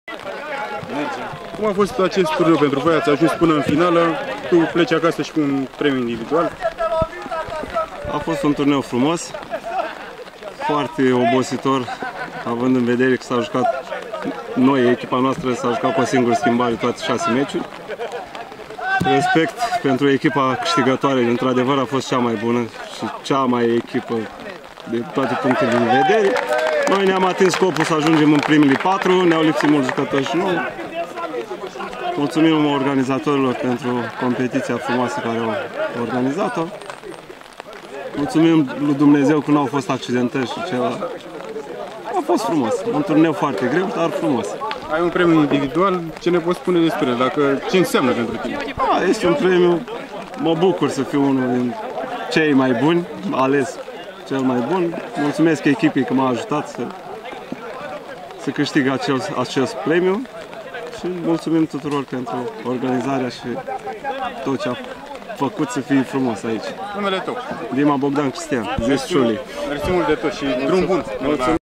Interviu - Bogdan Dima, jucator Mary Tour Iasi, locul 2 Old Boys, Severin
Interviu - Bogdan Dima „Shuly”, jucător Mary Tour Iași, locul 2 la Campionatul Național de Old Boys, Drobeta Turnu Severin 2019.

Shuly a fost declarat „Cel mai bun jucător” al competiției la care au participat 16 echipe.